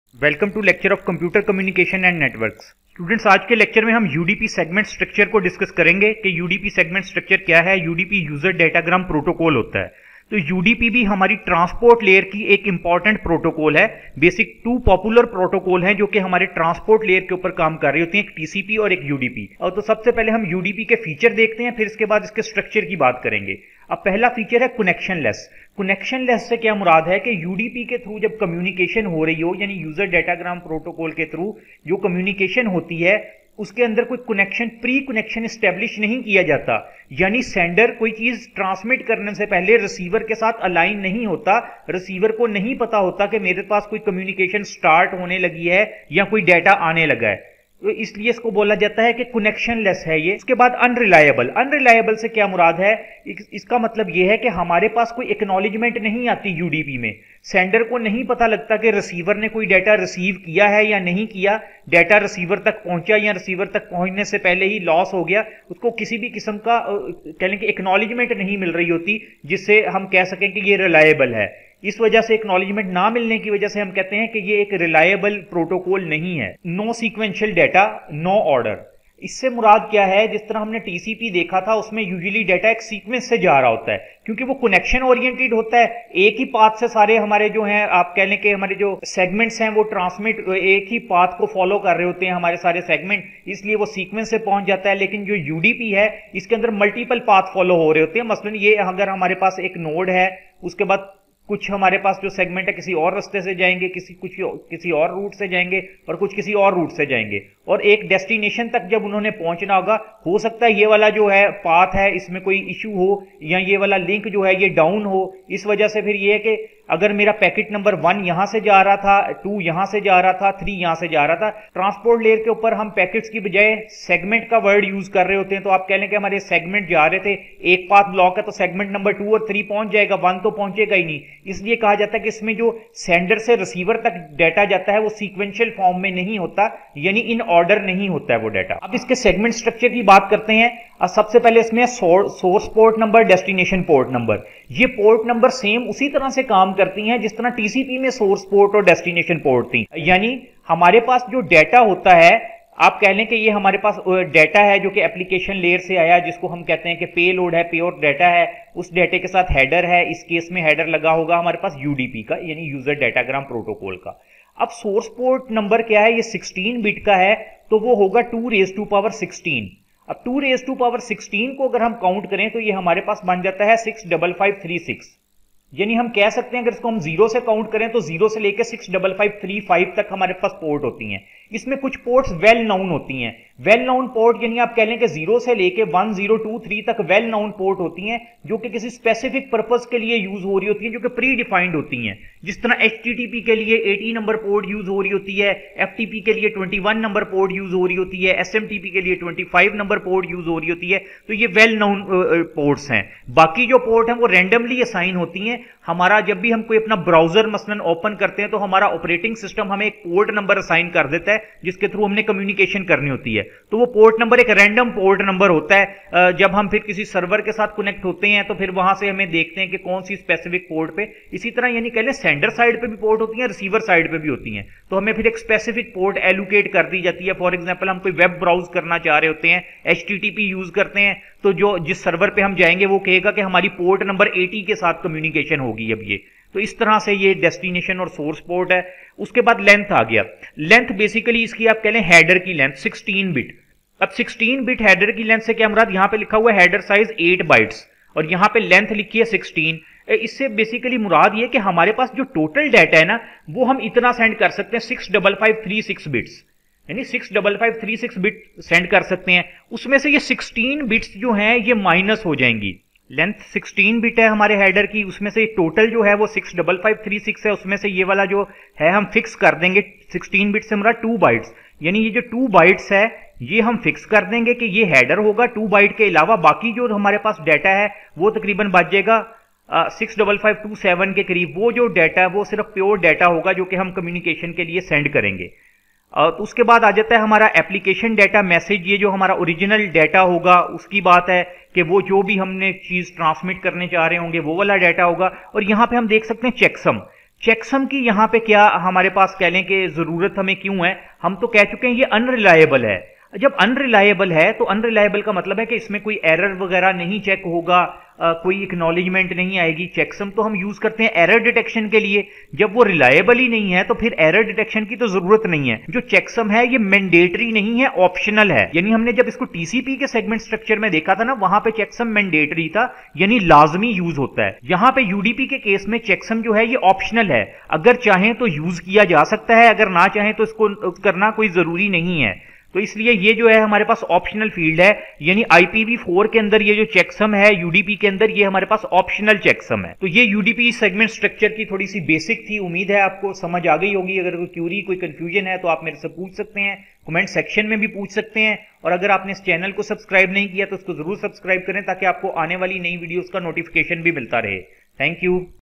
UDP Segment Structure Explained | user datagram protocol
UDP is user datagram protocol. In this video UDP segment structure is explained in detail and some features of UDP include; connectioneless, unreliable and no sequential data has also been explained. The field of UDP segment are
 source port numumer
destination port number
length
checksum